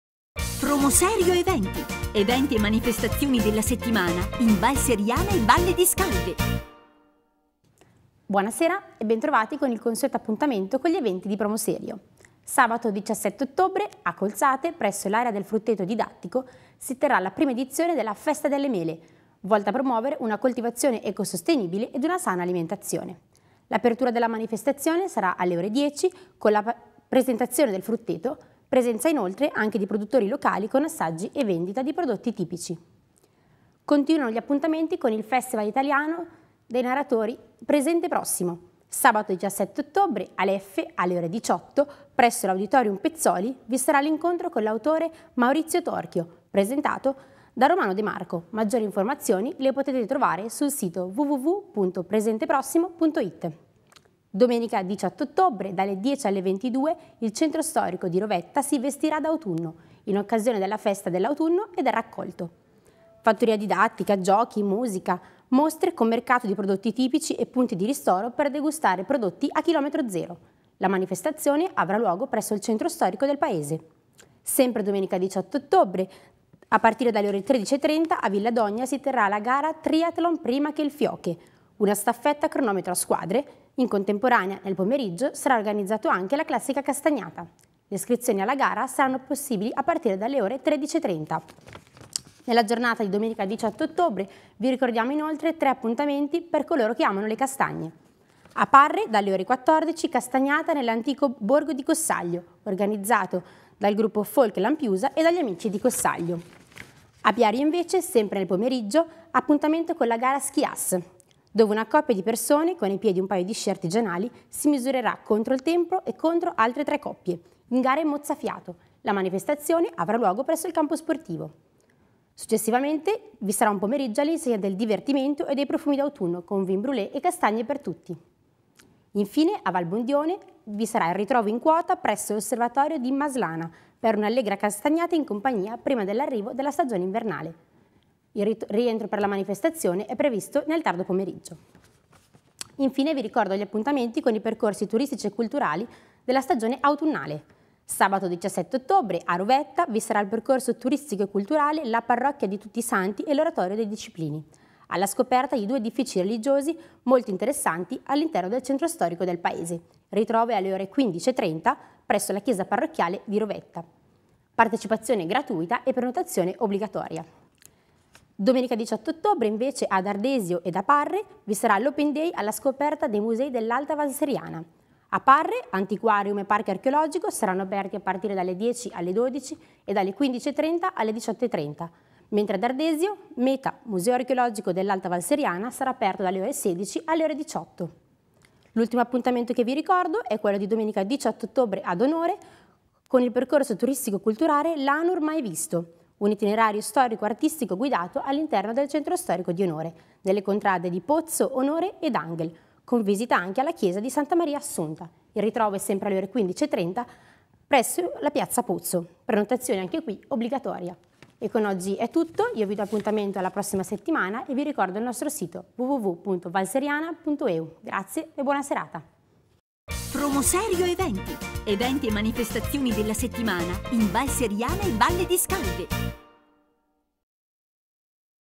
PromoSerio eventi dal 17 al 18 ottobre 2015
Gli eventi in Valle Seriana e Valle di Scalve.